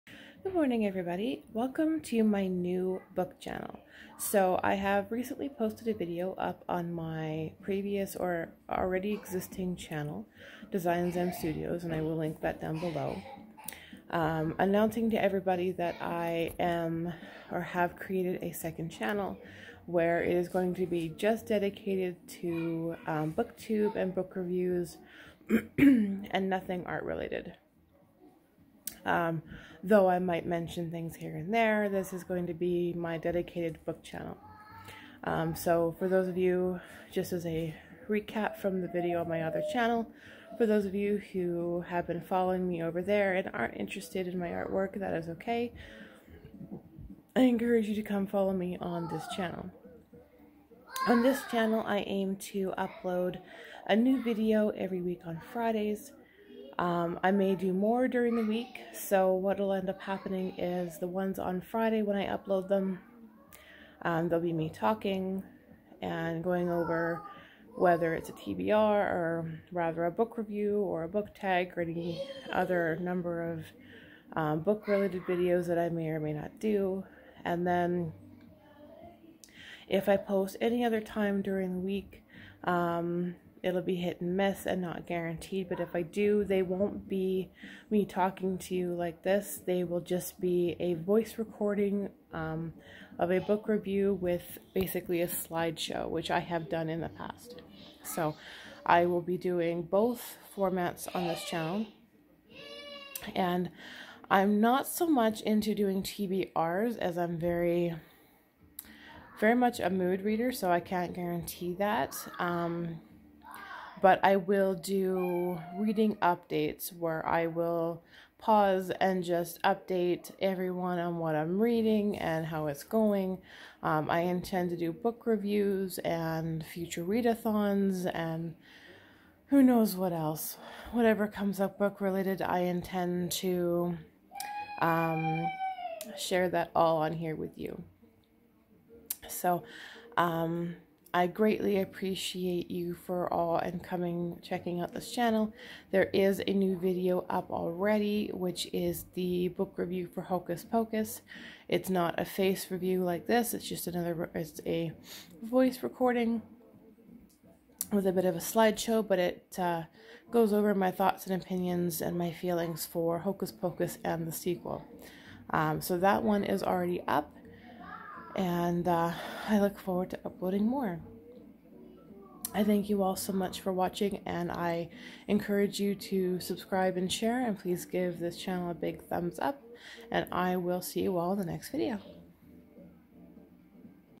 Skys book chat intro
Its been a very long two weeks! I realized i had uploaded the wrong video! This is the official intro video to this Channel sorry for the mix up!